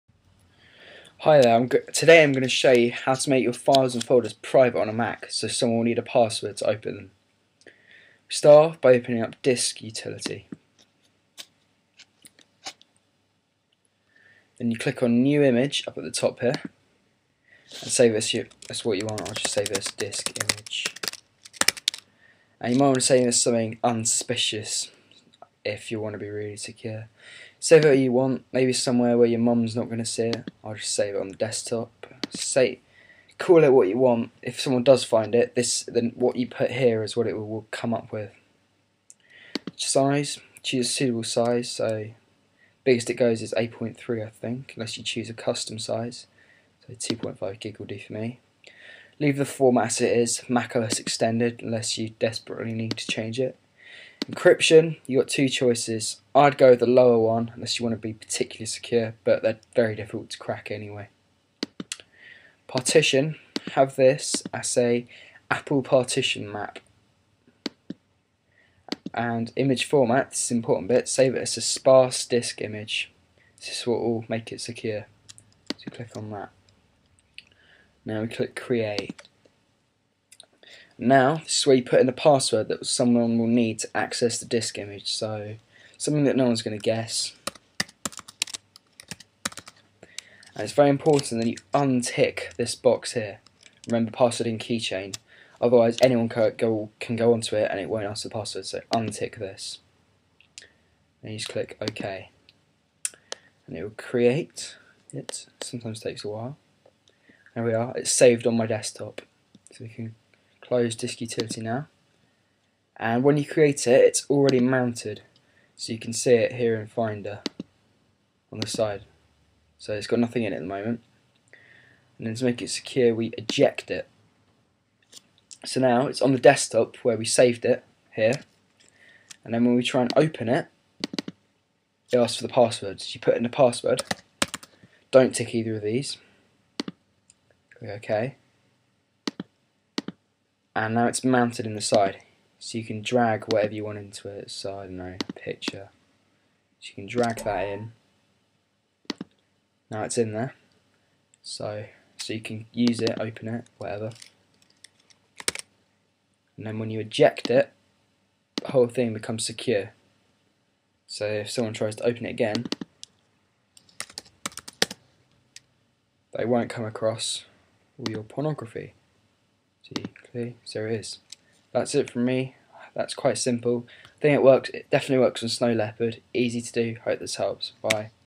How to Make Files and Folders Private on Mac OS X Snow Leopard
How to make files and folders password protected on a mac. Very easy really, would a appreciate a like, rating comment or whatever. This is my first video!